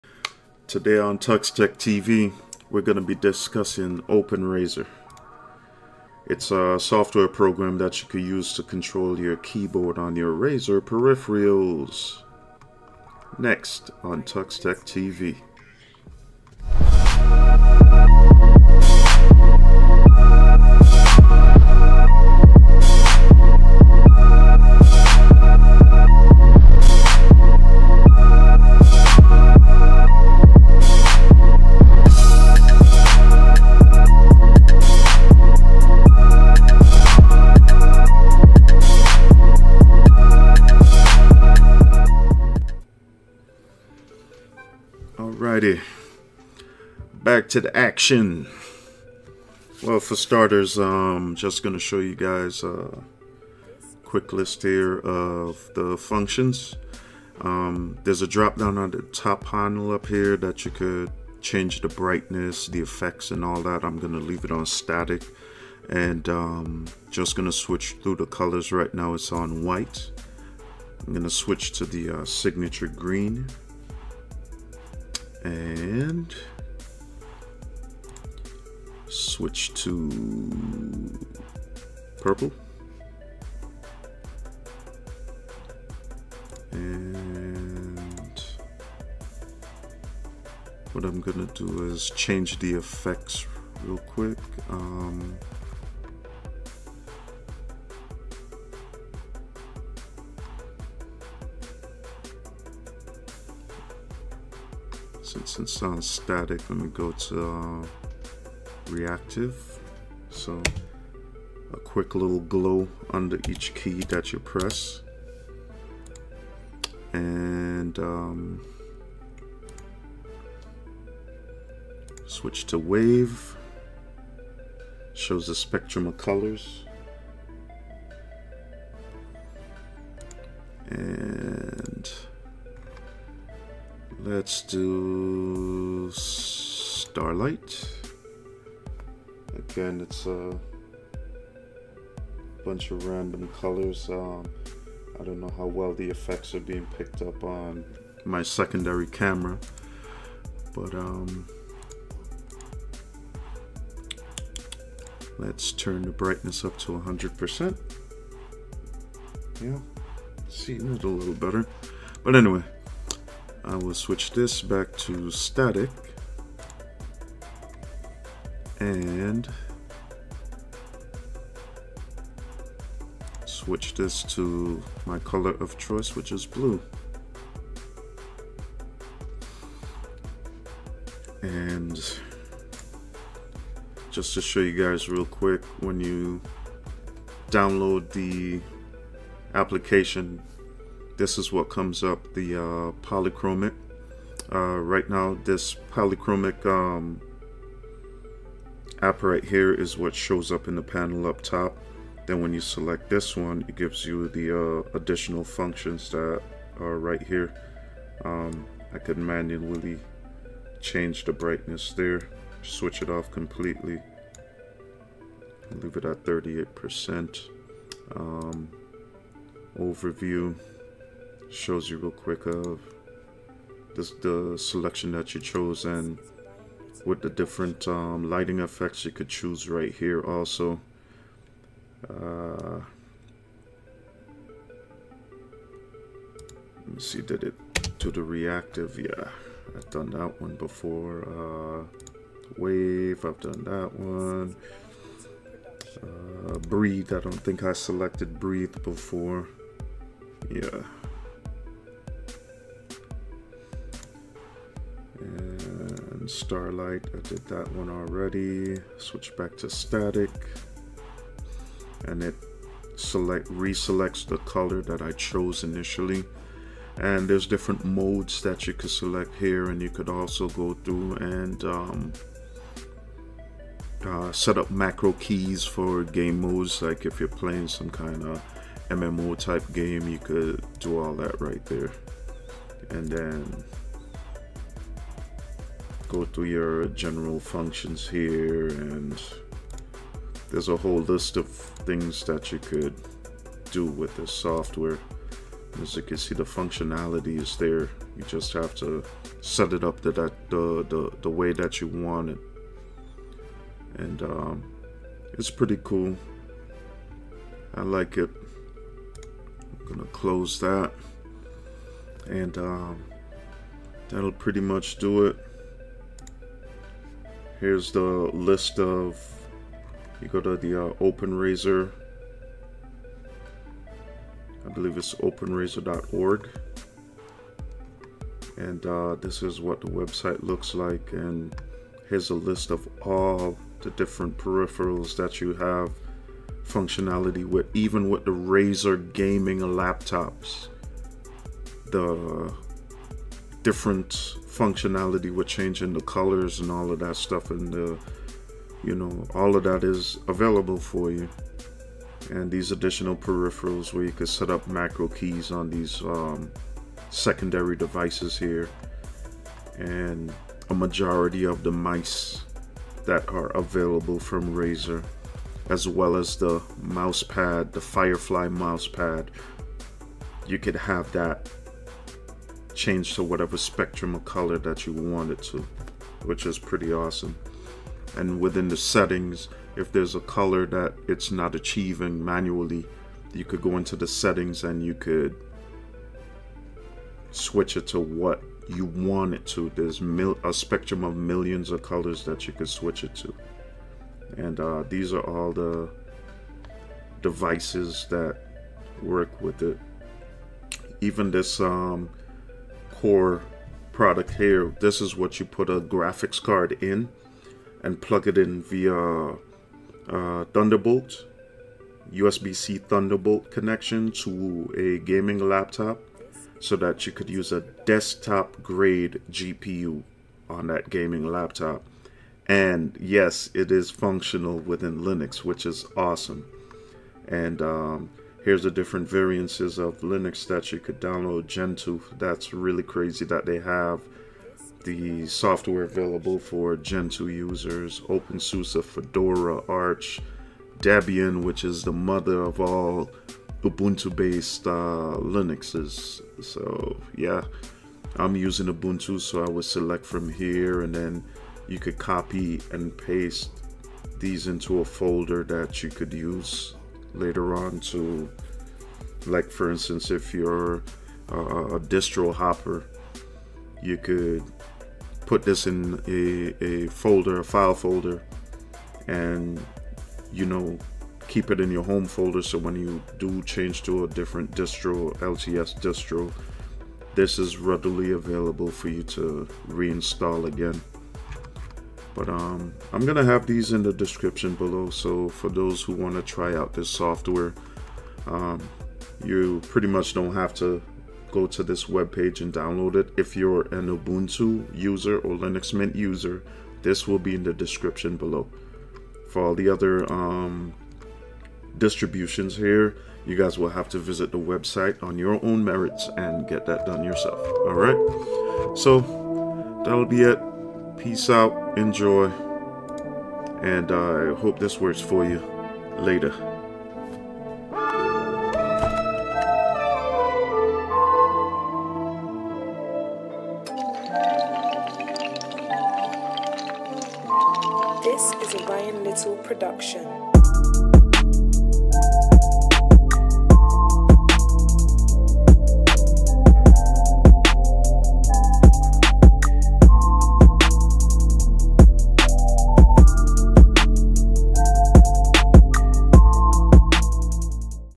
Razer Driver RGB Keyboard installs video.
PC DELL OPTIPLEX 990
The driver doesn't seem to work in Linux Mint 19.1

UBUNTU PPA:
sudo add-apt-repository ppa:openrazer/stable
sudo apt update
sudo apt install openrazer-meta

finish Open razer install with polychromatic:

echo "deb xenial main" | sudo tee -a /etc/apt/sources.list.d/polychromatic.list
sudo apt-key adv --recv-key --keyserver keyserver.ubuntu.com 96B9CD7C22E2C8C5
sudo apt-get update
sudo apt-get install polychromatic